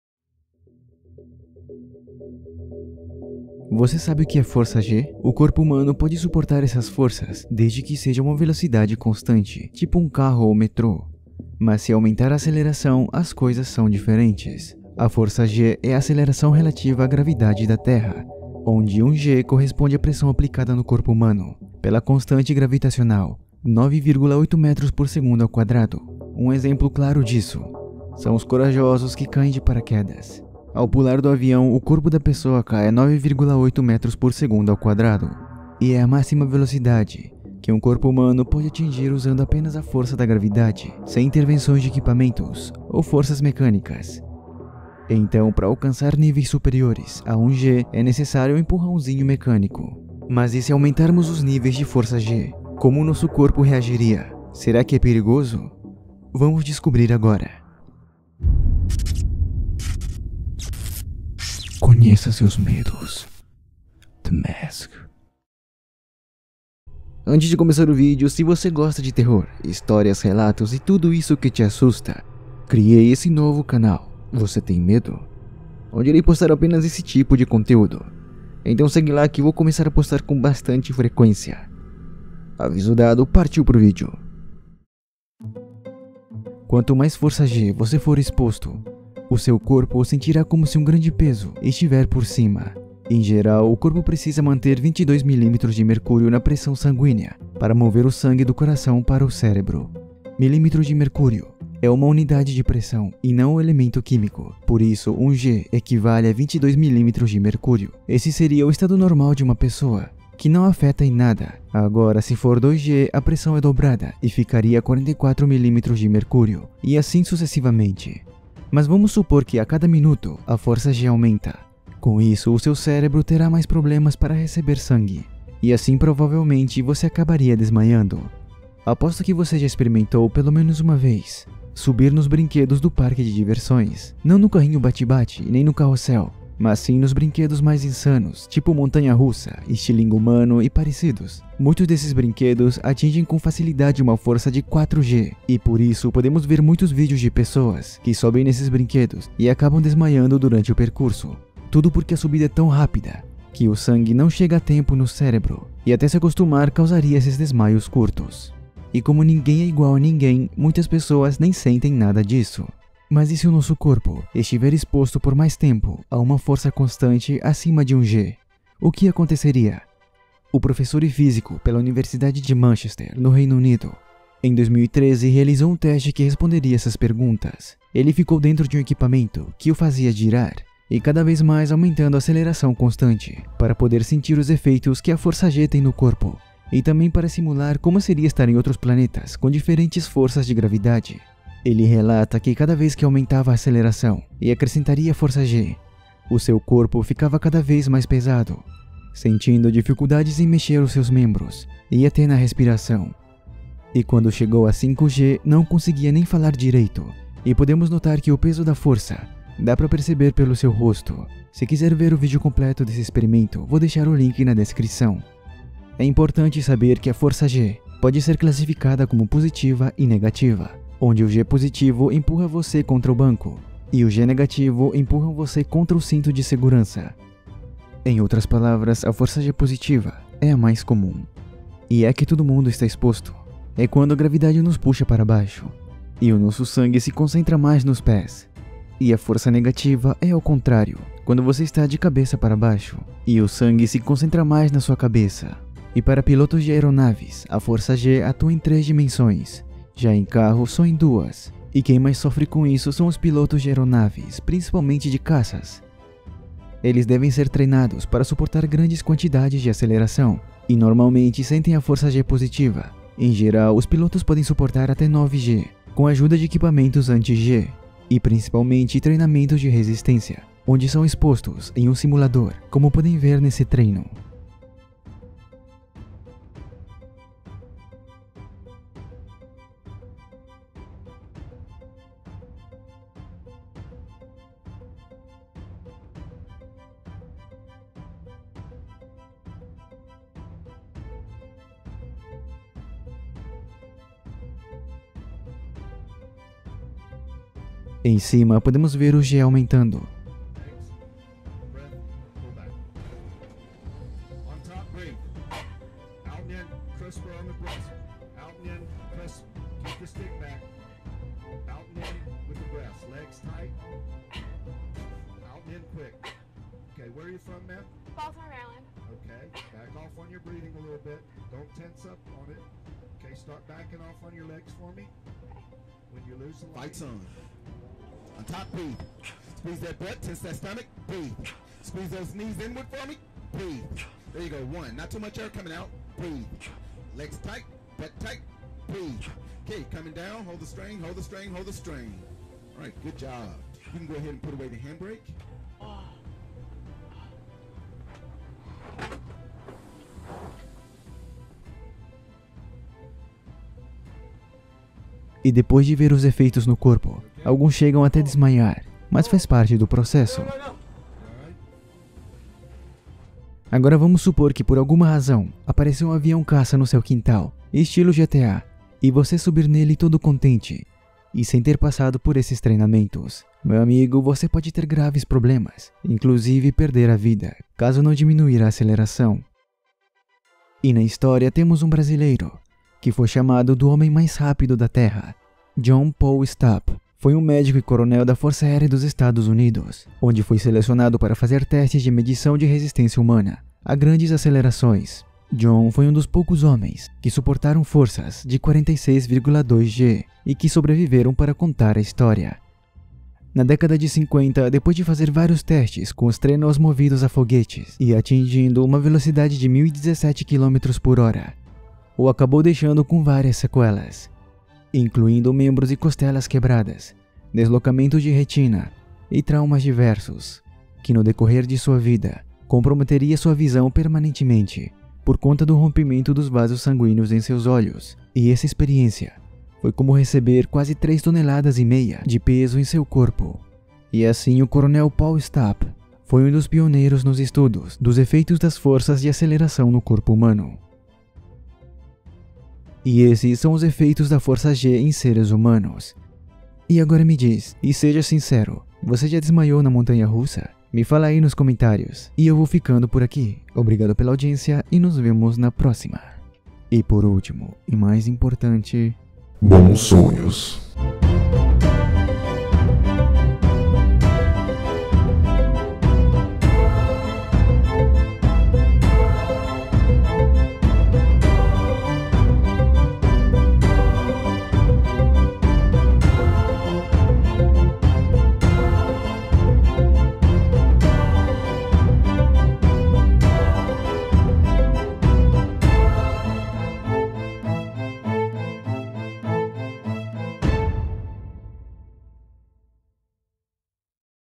Efeitos da FORÇA G no corpo humano
Você já deve ter estudado na escola sobre a força da gravidade do planeta Terra, a média da força G é 9,8 metros por segundo ao quadrado, e é nessa média que nos estamos acostumados... mas e se a gravidade aumentasse a força? como o nosso corpo reagiria? será perigoso? Vamos ver os efeitos nesse vídeo!